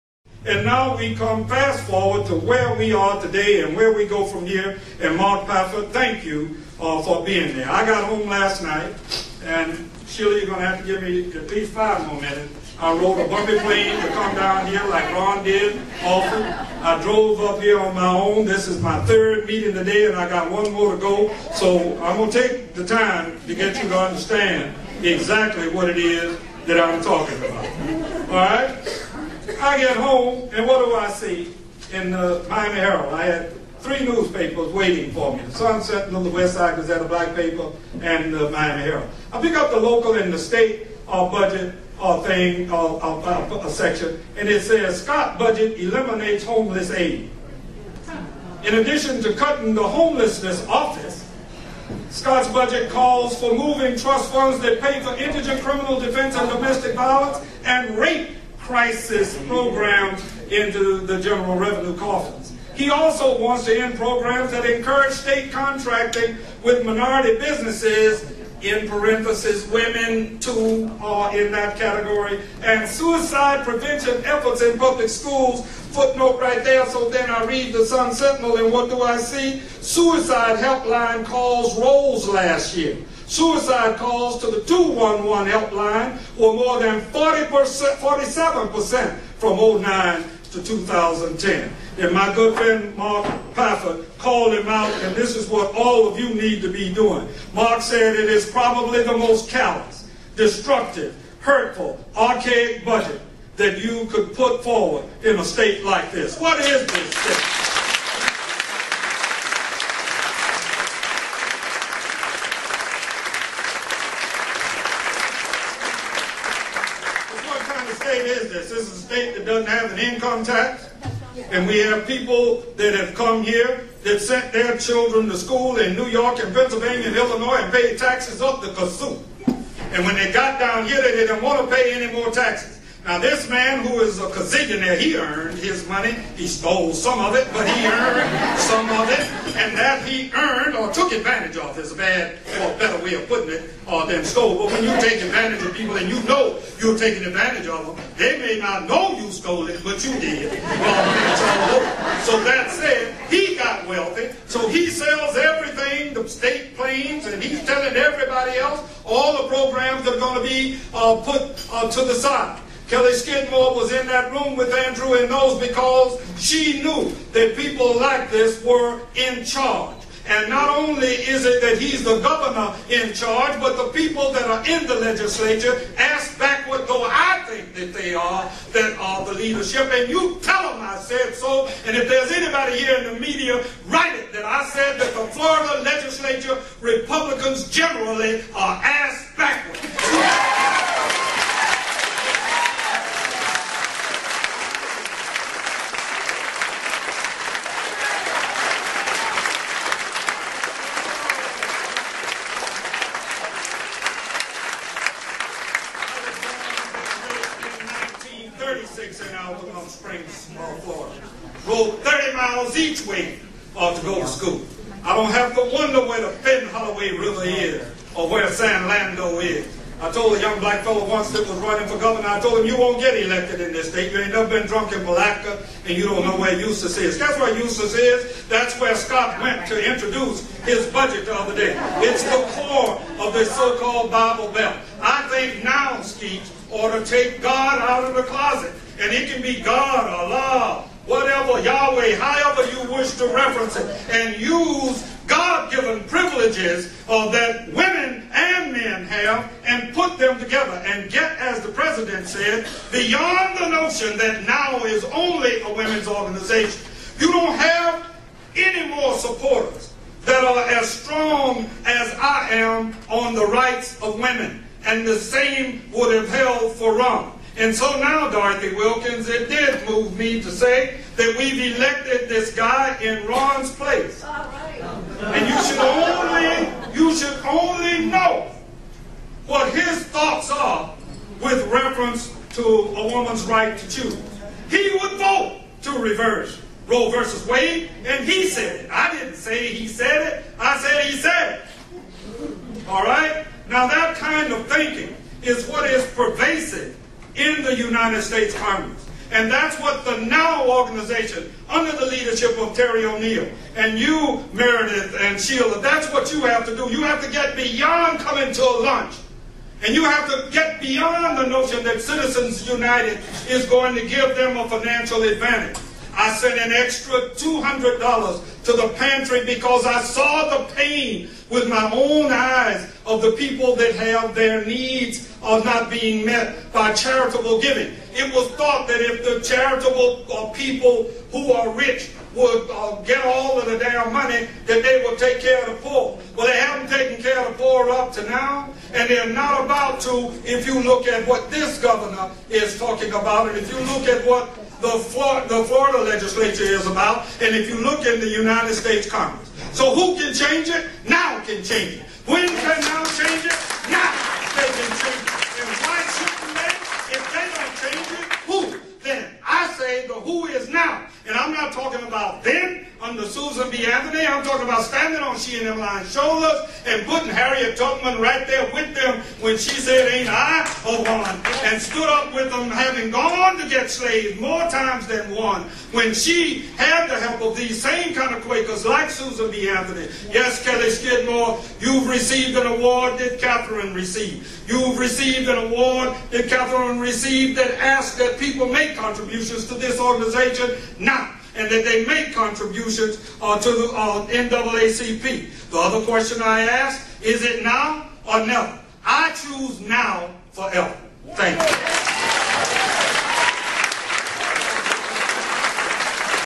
Congressman Alcee Hastings Speaks Before Palm Beach County's National Organization for Women (NOW)
Congressman Alcee L. Hastings Speaks at the Palm Beach County National Organization for Women's 38th Annual Susan B. Anthony Feminist of the Year Award Luncheon on February 12, 2011.